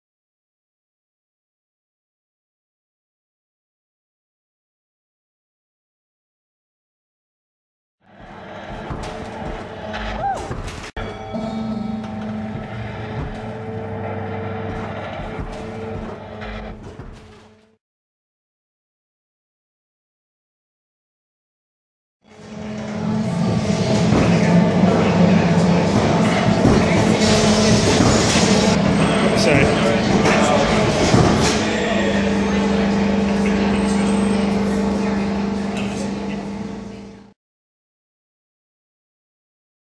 Test Crash Dummy Simulation With Oculus Rift
Hands-on with the NRMA Insurance Research Centre's virtual crash test dummy at Sydney's Crashed Car Showroom.